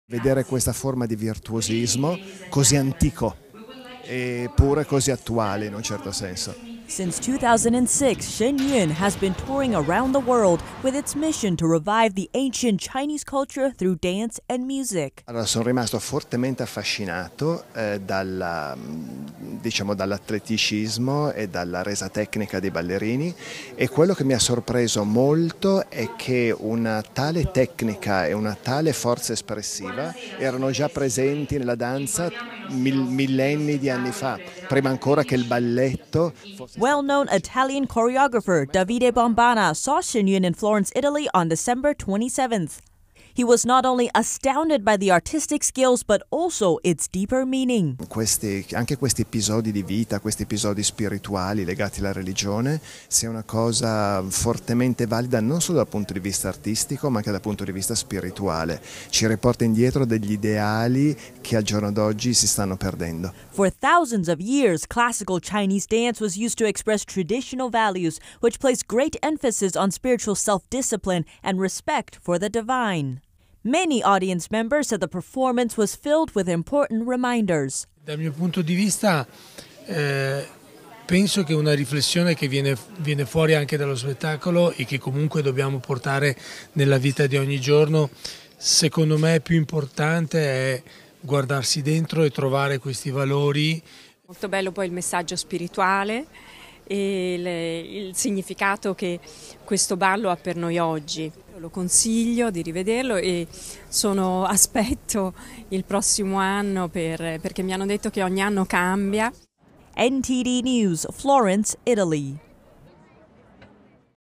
Shen Yun reminds us of important values, says choreographer I NTDTV
ShenYun has been touring around the world with its mission to revive ancient Chinese culture through dance and music.

A well-known Italian choreographer Davide Bombana in Italy said he was astounded by the choreography and the dance techniques.

“I was very fascinated by the art, the superb skills and the technical performance of the dancers.”

“To see this form of virtuosity, so ancient and yet in a sense so fresh, is for me an incredible surprise.”

For thousands of years, classical Chinese dance was used to express traditional values which place great emphasis on spiritual self-discipline and respect for the divine.

Many audience members said the performance was filled with important reminders.

“For me, this performance allows us to examine ourselves in our daily lives,” said Pierangelo Conte, the artistic director of Florence Opera. “I think it is more important to look inward and find these values. And this performance can remind us of that.”